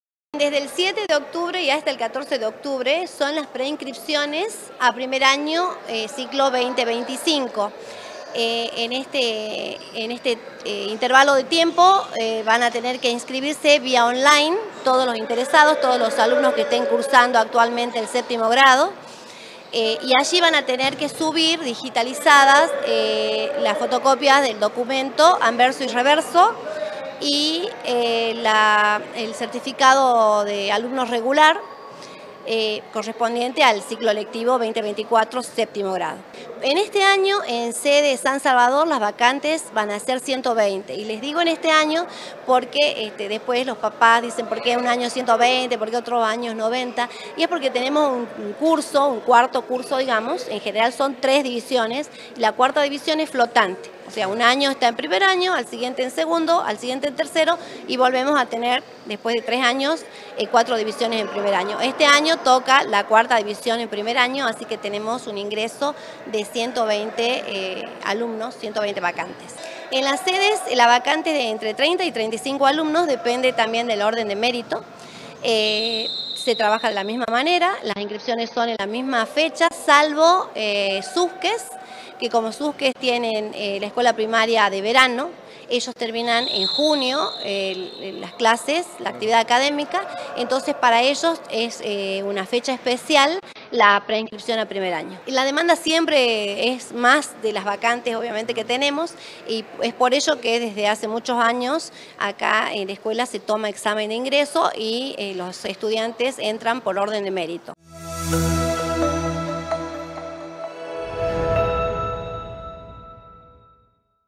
Ingreso a 1er año: comenzó la preinscripción en la Escuela de Minas
Del 7 al 14 de octubre se están realizando las preinscripciones para el ingreso a 1er año en la Escuela de Minas. Hay 120 vacantes en San Salvador de Jujuy.